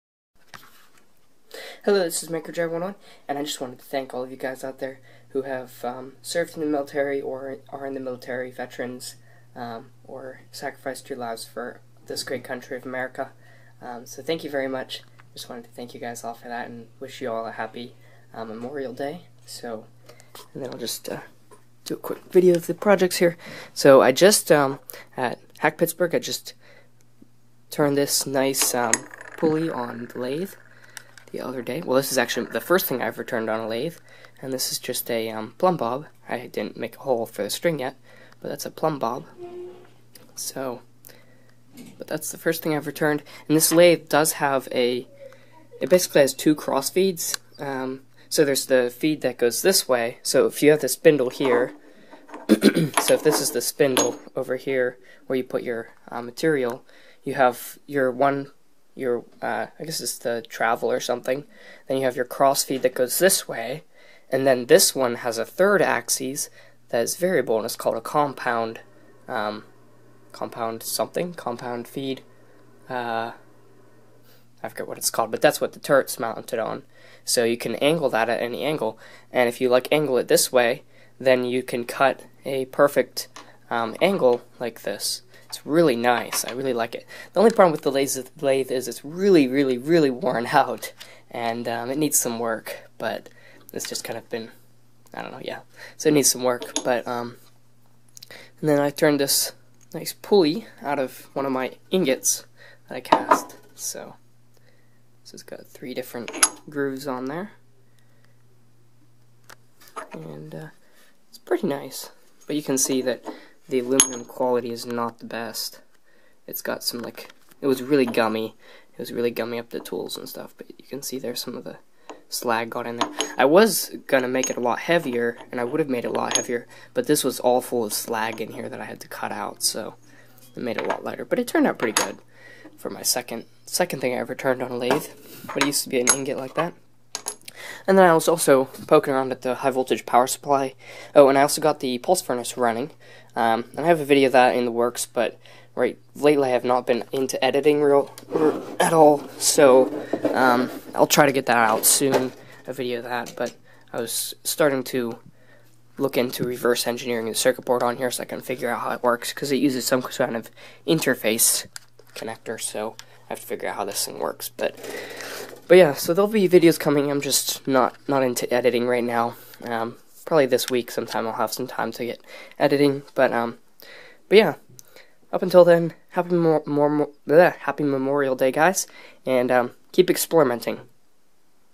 Happy Memorial Day, Turned Some Things on the Lathe, High Voltage Power Supply
Just a quick video!  Enjoy your memorial day!  :)